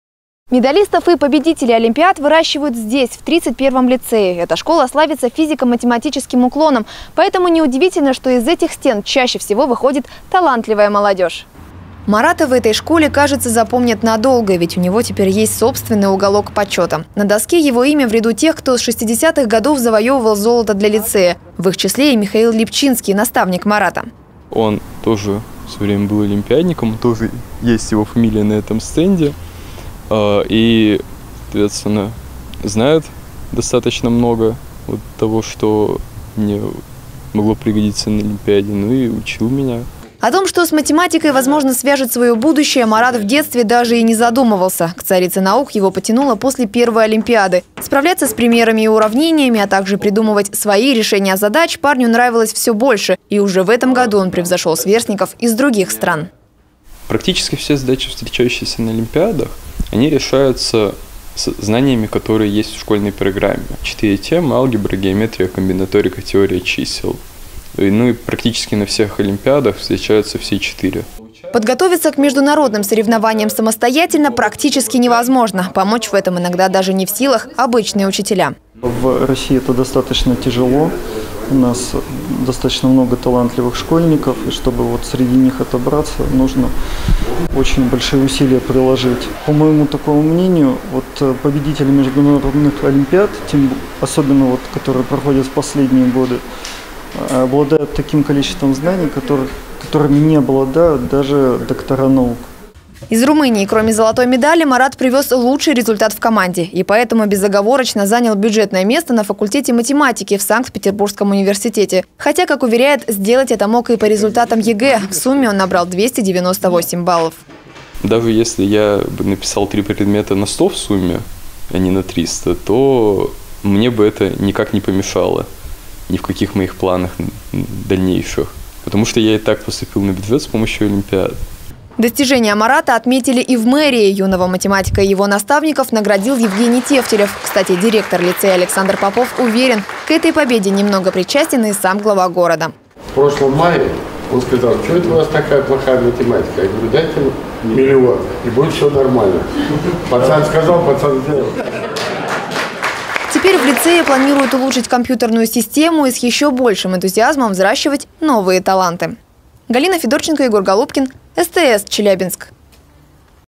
Математика на вес золота
Школьник из Челябинска стал победителем международной олимпиады в Румынии